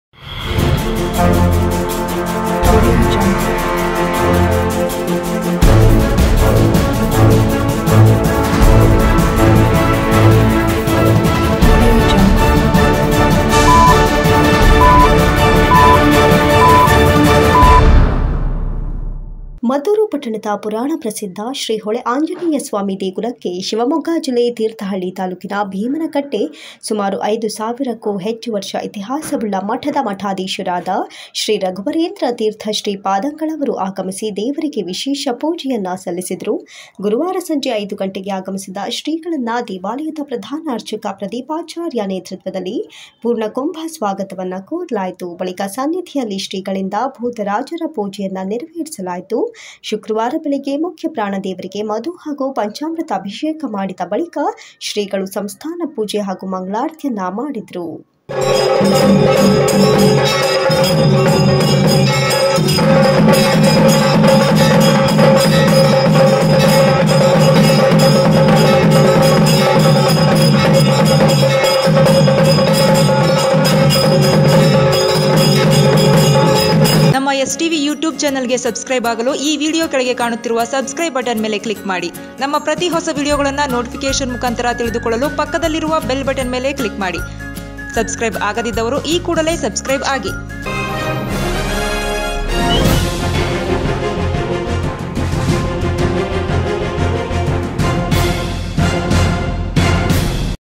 S.Tv News Mandya
*ಶ್ರೀ ಹೊಳೆ ಆಂಜನೇಯಸ್ವಾಮಿ ದೇಗುಲಕ್ಕೆ ಶಿವಮೊಗ್ಗ ಜಿಲ್ಲೆ ತೀರ್ಥಹಳ್ಳಿಯ ಶ್ರೀ ರಾಘವೇಂದ್ರ ತಿರ್ಥ ಶ್ರೀ ಪಾದಂಗಳವರು ಆಗಮಿಸಿ ಪೂಜೆ.

*ಶ್ರೀ ಹೊಳೆ ಆಂಜನೇಯಸ್ವಾಮಿ ದೇಗುಲದ ಸನ್ನಿಧಿಯಲ್ಲಿ ಶ್ರೀಗಳಿಂದ ಭೂತರಾಜರ ಪೂಜೆ ನೆರವೇರಿಸಿದರು.